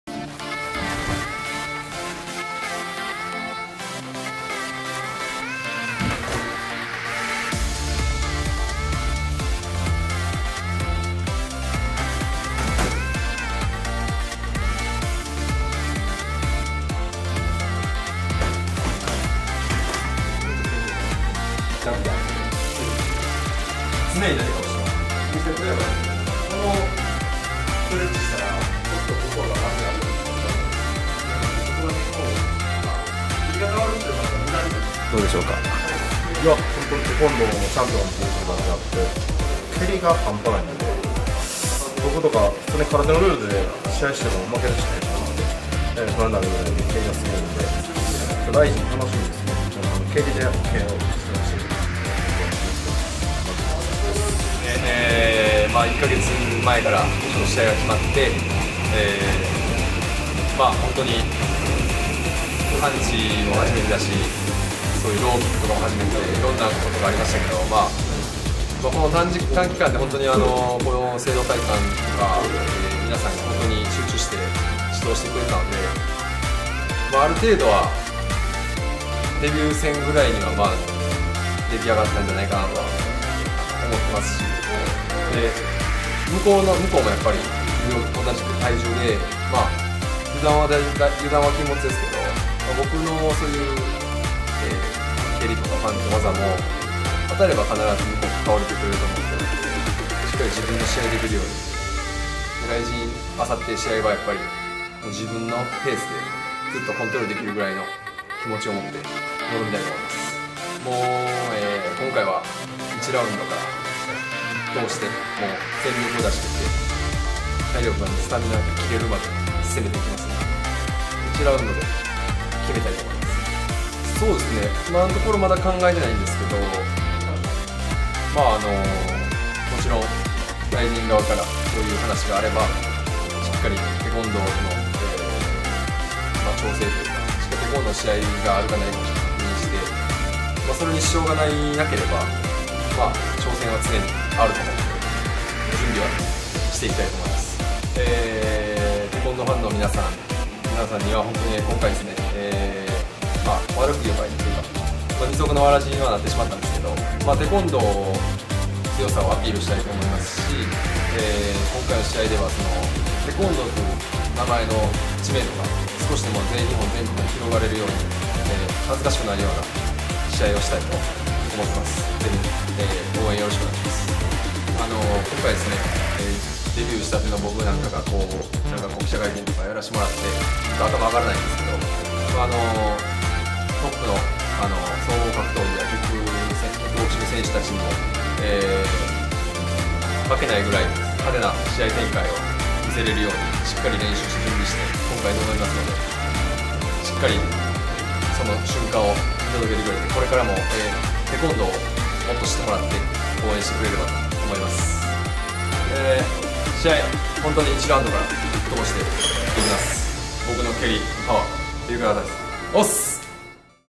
RIZIN25｜最終調整が終了！
Rizin25出場が決定したテコンドー全日本9連覇中の絶対王者 江畑秀範の最終調整とインタビュー